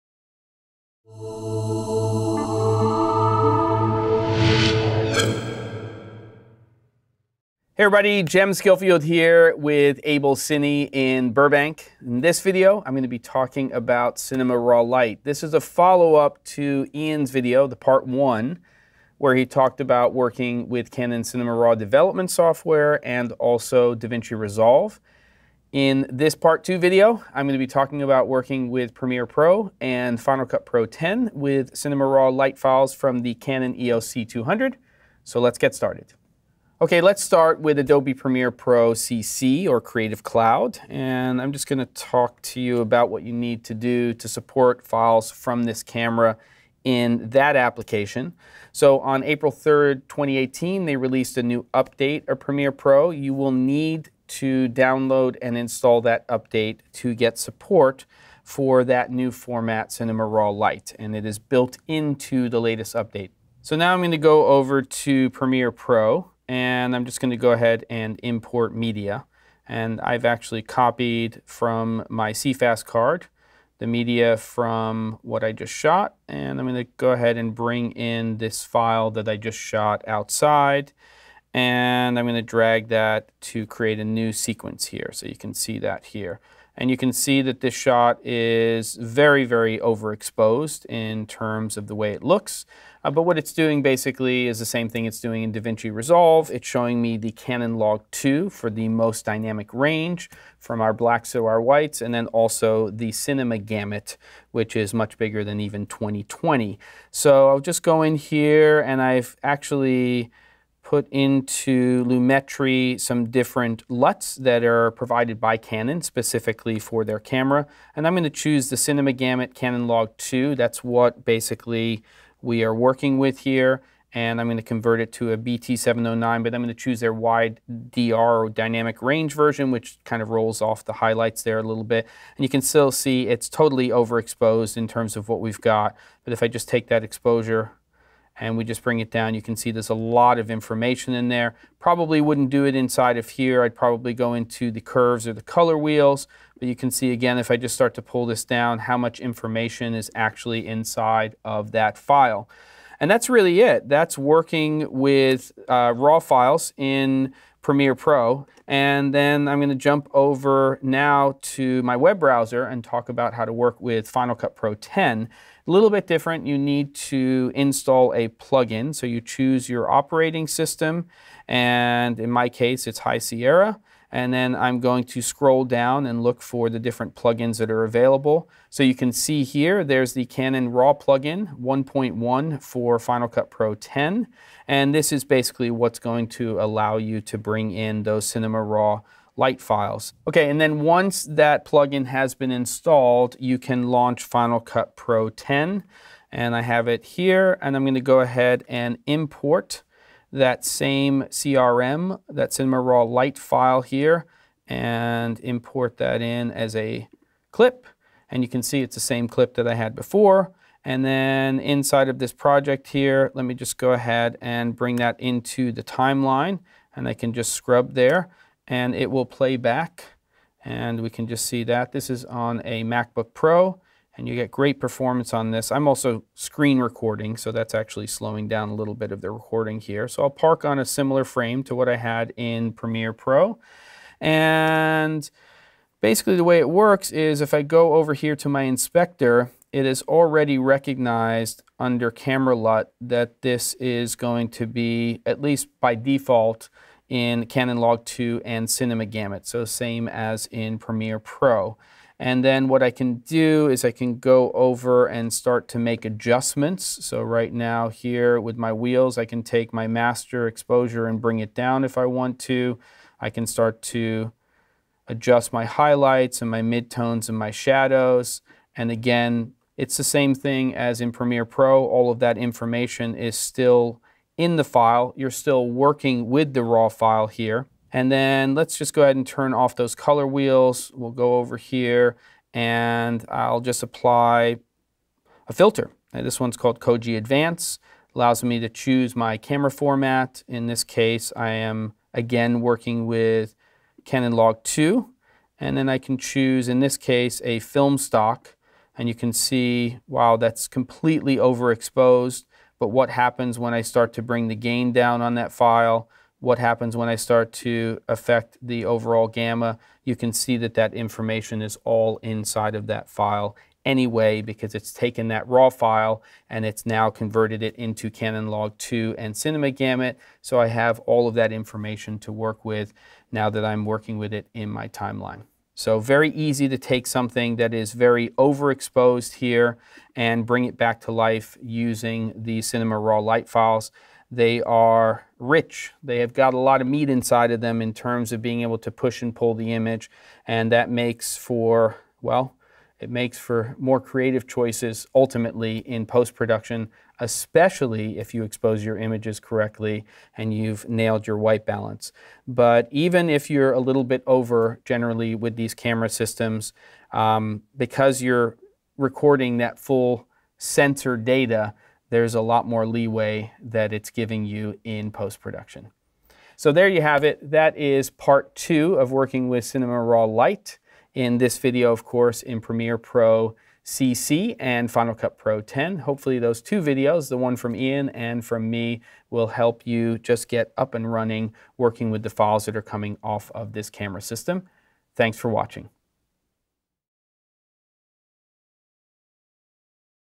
At the Bench: Canon C200 Cinema RAW Light Workflow (Part 2)
Earlier this year, Ian covered two workflows using Canon's own Cinema RAW Development software and DaVinci Resolve. Now that Cinema RAW Light is supported in Premiere Pro and FCP X, Jem Schofield shot a follow up blog demonstrating how to access and work with these files in the popular NLEs.